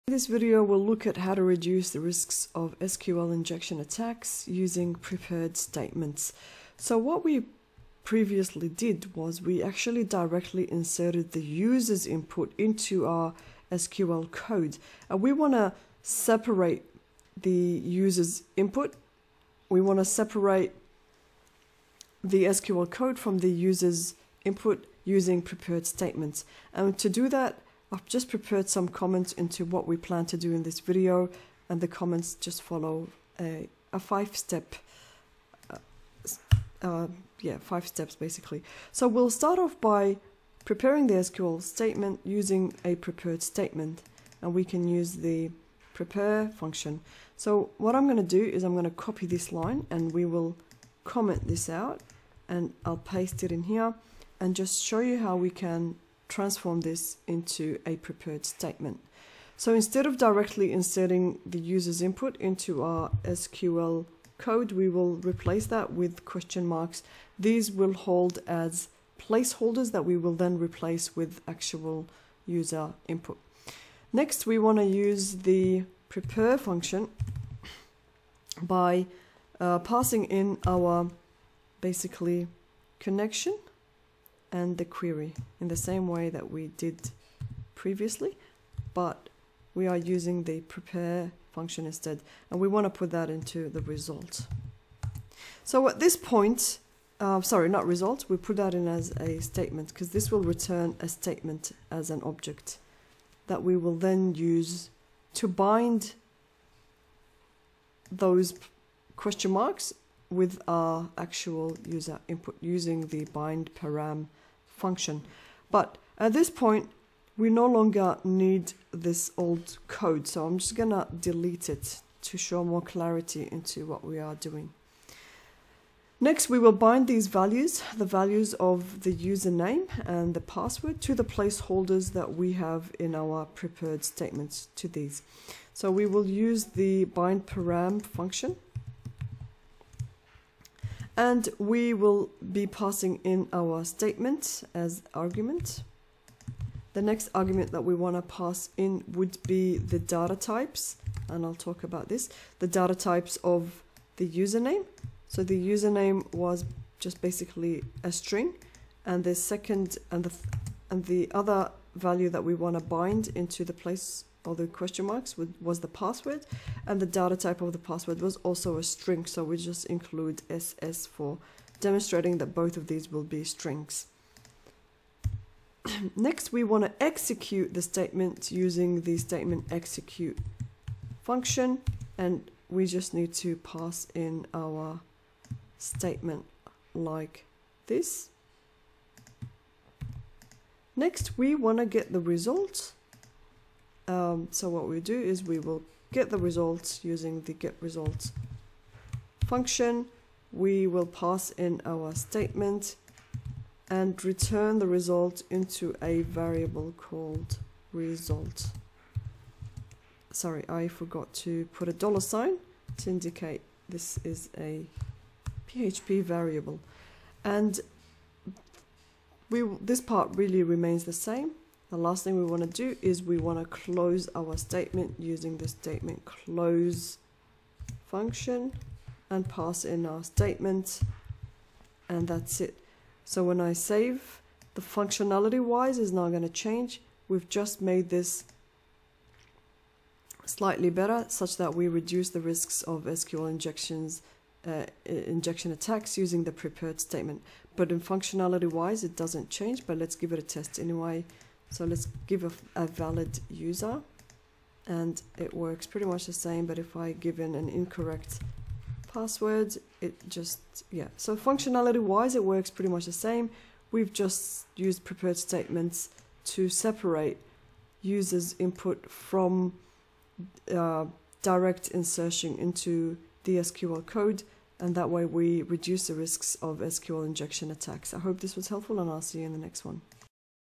PHP and MySQL 9: Reduce the risks of SQL injection attacks using prepared statements.
Reduce the risks of SQL injection attacks using prepared statements.